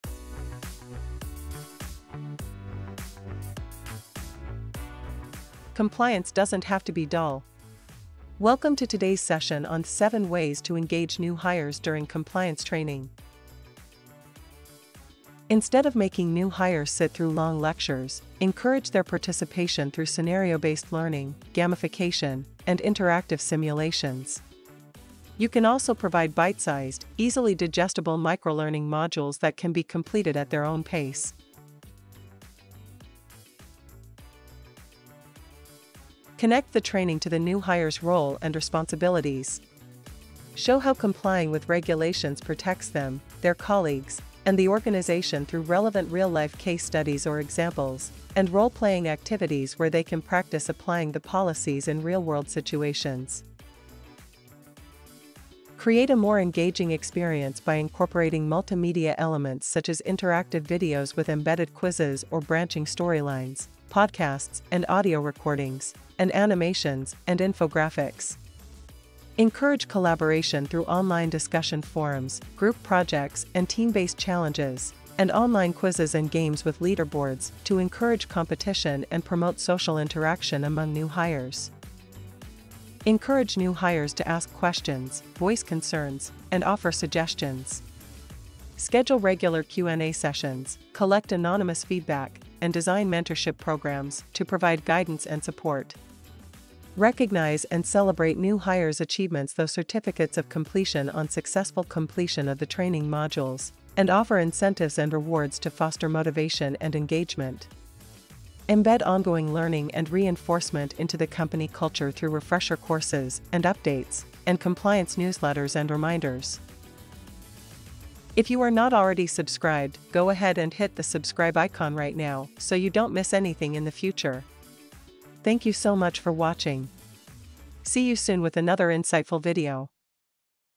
7 Engaging Strategies for New Hires
Discover 7 dynamic strategies to captivate new hires during compliance training. Dive into interactive learning experiences that foster engagement and retention. Personalize the journey with relevant content tailored to individual needs. Enrich the learning process with multimedia elements for a stimulating and immersive experience. Foster collaboration and knowledge sharing through social learning platforms, promoting a culture of continuous improvement. Provide valuable feedback mechanisms to enhance performance and ensure comprehension. Celebrate milestones and achievements with recognition programs, motivating employees to excel. Embrace a culture of ongoing learning, empowering new hires to evolve and adapt in a dynamic environment. Elevate your compliance training with these innovative approaches to maximize engagement and drive success.